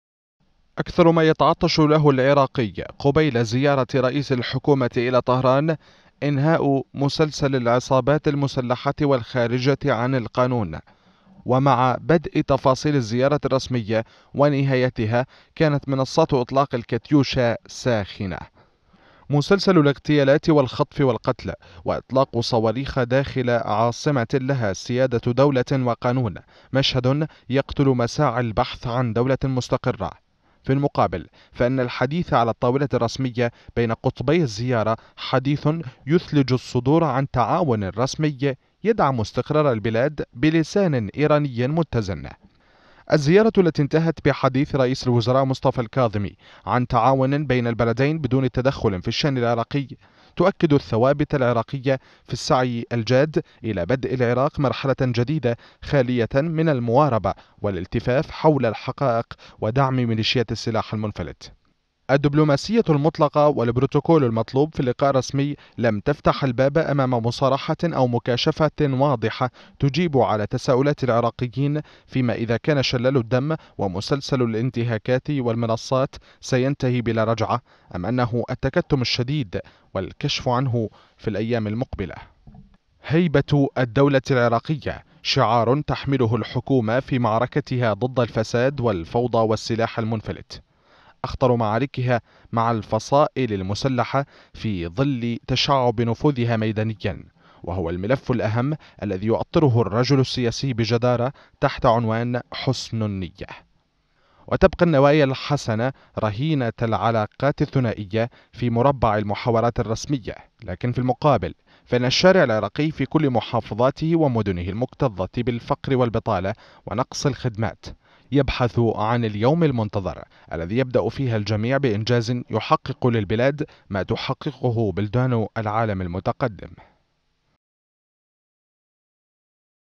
هل ينتهي مسلسل عصابات السلاح بعد زيارة الكاظمي لإيران؟ | تقرير
هل ينتهي مسلسل عصابات السلاح بعد زيارة إيران؟
لا ينفك العراقي من التفكير في أيامه المقبلة، وانتظار وصول البلاد إلى بر الأمان، وإنهاء أشكال العنف والإرهاب، سنوات عجاف يتطلع الشعب العراقي اليوم بعدها إلى تحقيق نتائج فاعلة بعيد الزيارة الرسمية إلى طهران.